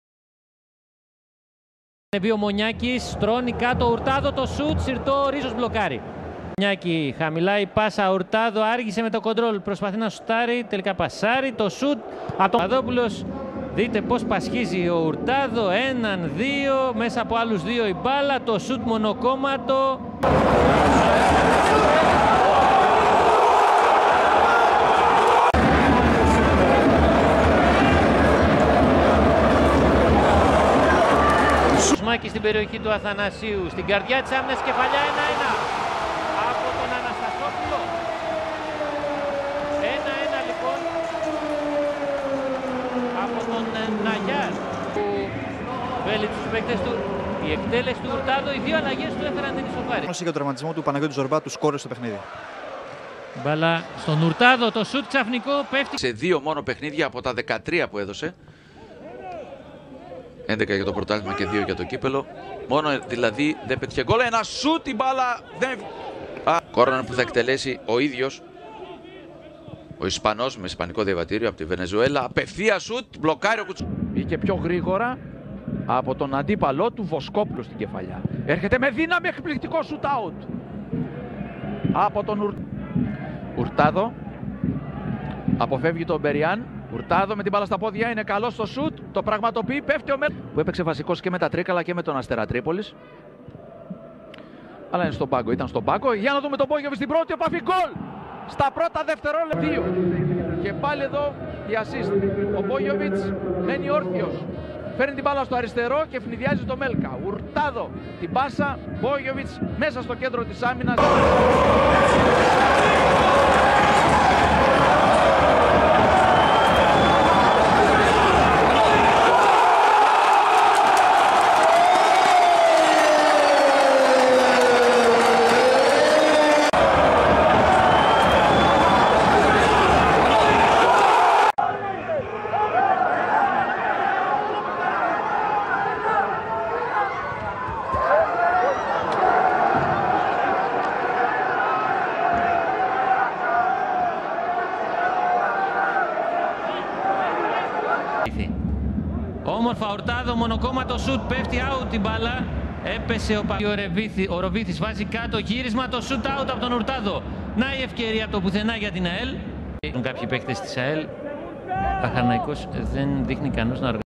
Πολ Ουρτάδο ΑΕΛ | Pol Hurtado AEL
Φάσεις από τον Πολ Ουρτάδο με τη φανέλα της ΑΕΛ
ευχαριστώ τους Mike Loupnikis, MonaxaLarisa και saturn3108